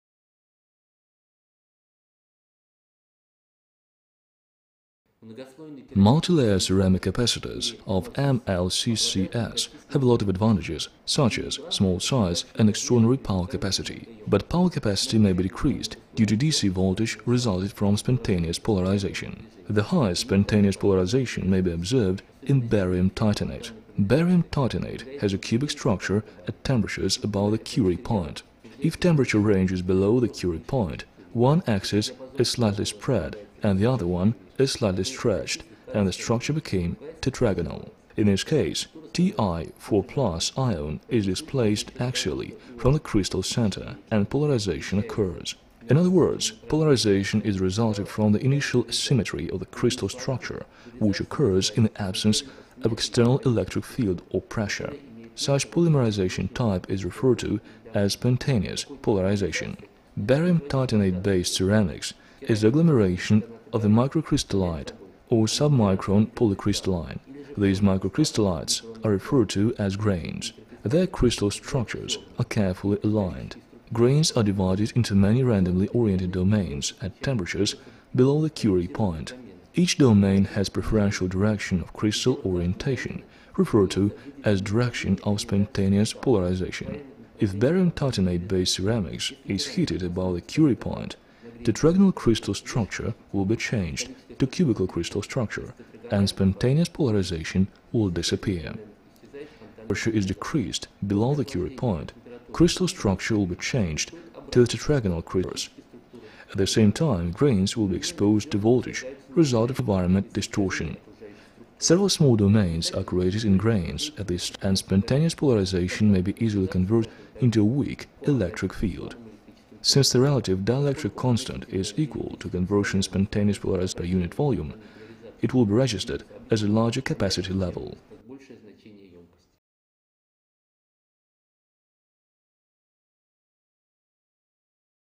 BaTiO3 Features. Spontaneous Polarization
BaTiO3 Features. Spontaneous Polarization       Multilayer ceramic capacitors or MLCCs have a lot of advantages such as and small size and extraordinary power capacity. But power capacity may be decreased due to DC voltage resulted from a spontaneous polarization.       The highest spontaneous polarization may be observed in barium titanate. Barium titanate has a cubic structure at temperatures above the Curie point. If temperature range is below the Curie point, one axis is slightly spreads and the other one is slightly stretched and the structure became tetragonal.       In this case, Ti4+ ion is displaced axially from the crystal center and polarization occurs. In other words, polarization is resulted from the initial asymmetry of the crystal structure which occurs in the absence of external electric field or pressure. Such polymerization type is referred to as spontaneous polarization.       Barium titanate based ceramics is the agglomeration of microcrystallite or submicron polycrystalline. These microcrystallites are referred to as grains. Their crystal structures are carefully aligned. Grains are divided into many randomly oriented domains at temperatures below the Curie point.       Each domain has preferential direction of crystal orientation referred to as direction of spontaneous polarization. If barium titanate based ceramics is heated above the Curie point, tetragonal crystal structure will be changed to cubical crystal structure and spontaneous polarization will disappear.       When the temperature is decreased below the Curie point, crystal structure will be changed to tetragonal crystal in reverse. At the same time grains will be exposed to voltage resulted from environment distortion. Several small domains are created in grains at this stage and spontaneous polarization may be easily converted into a weak electric field.       Since the relative dielectric constant is equal to conversion spontaneous polarization per unit volume, it will be registered as a larger capacity level.